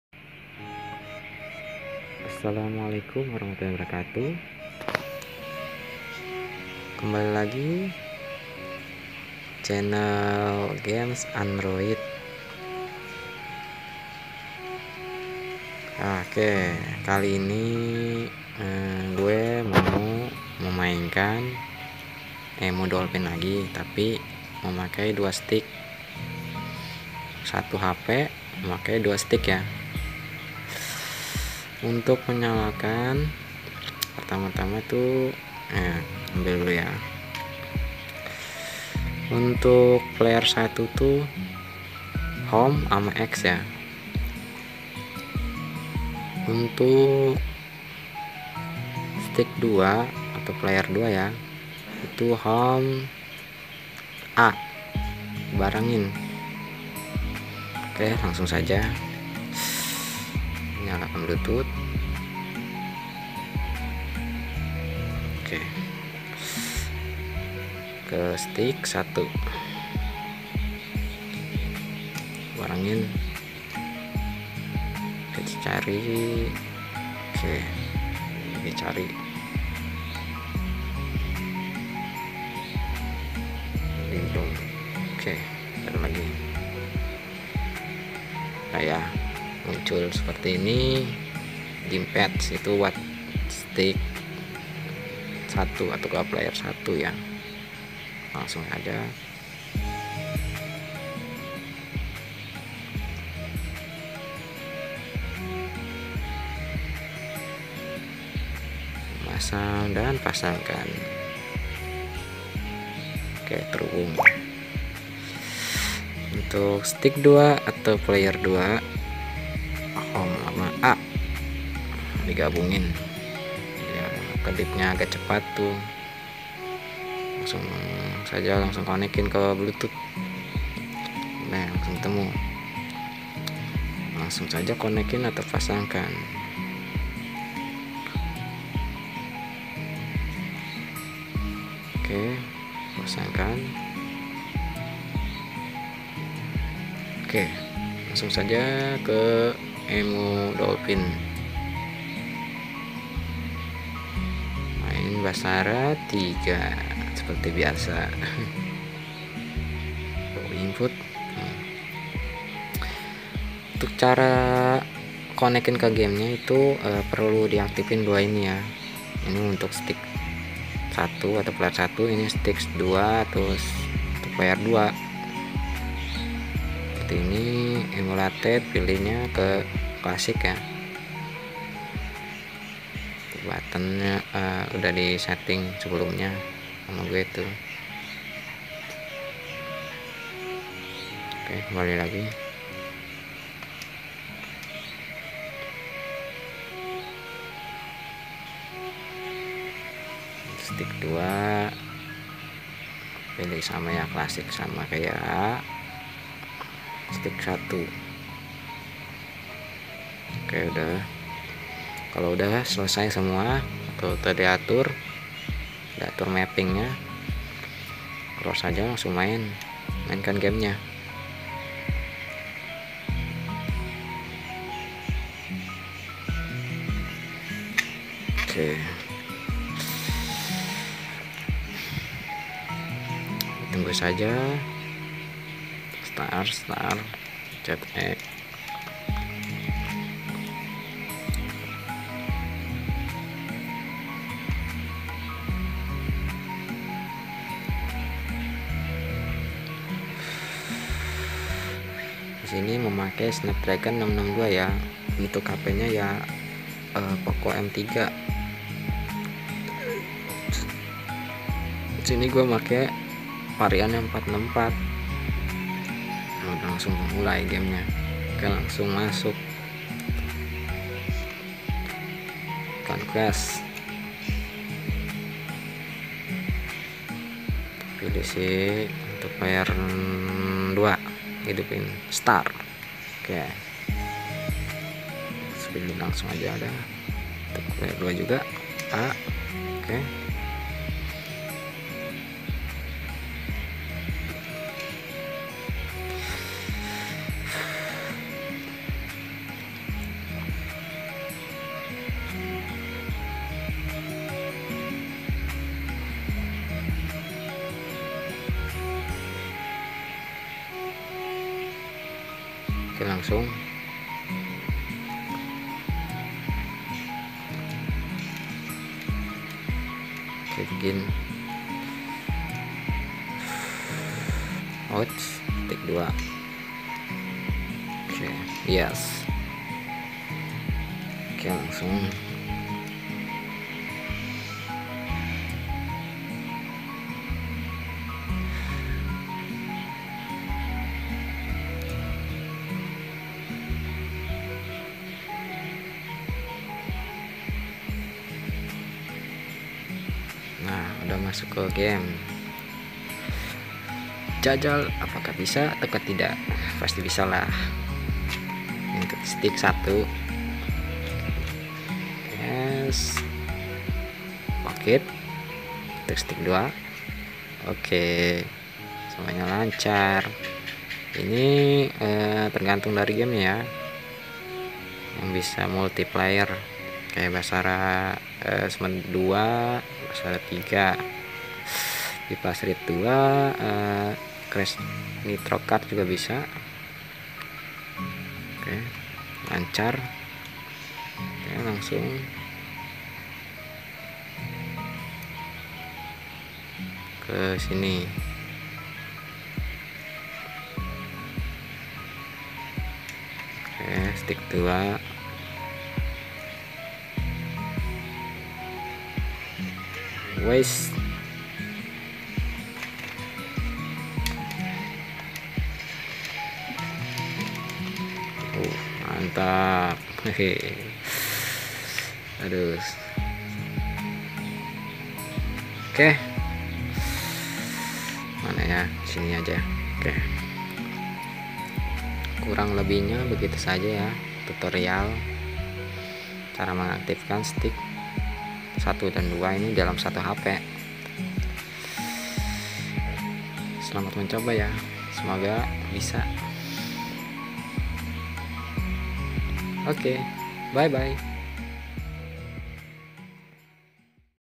Tut0rial multiplayer dolphin emulator
untuk cara ini sudah pasti worth it ya,asalkan dari game nya saja sudah di pastikan bisa multiplayer apakah tidak

kebanyakan dari game wii itu tidak support classic control,jadi usaha kan yang bisa classic control tanpa dari config control itu ya,harus manual seting

my phone
poco m3
snapdragon 662